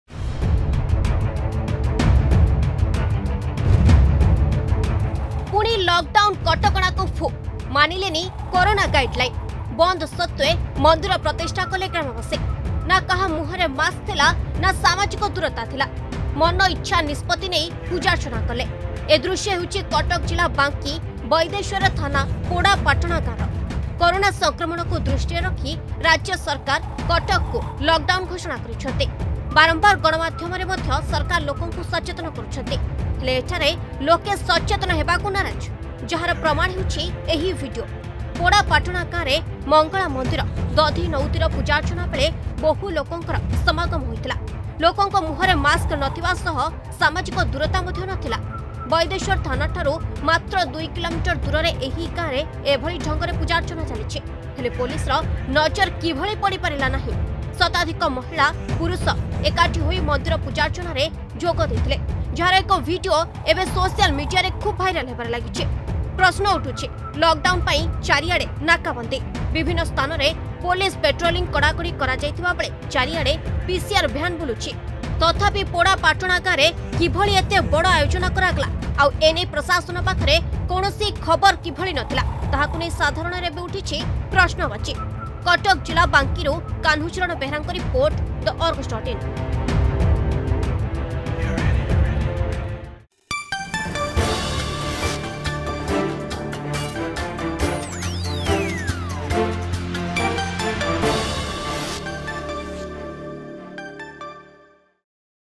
ପୁଣି ଲକ ଡାଉନ କଟକଣାକୁ ଫୁ... |TheArgus
ପୁଣି ଲକ ଡାଉନ କଟକଣାକୁ ଫୁ... ମାନିଲେନି କୋରୋନା ଗାଇଡଲାଇନ୍... ବନ୍ଦ ସତ୍ତ୍ୱେ ମନ୍ଦିର ପ୍ରତିଷ୍ଠା କଲେ ଗ୍ରାମବାସୀ... ନା କାହାର ମୁହଁରେ ମାସ୍କ ଥିଲା ନା ସାମାଜିକ ଦୂରତ୍ୱ ଥିଲା.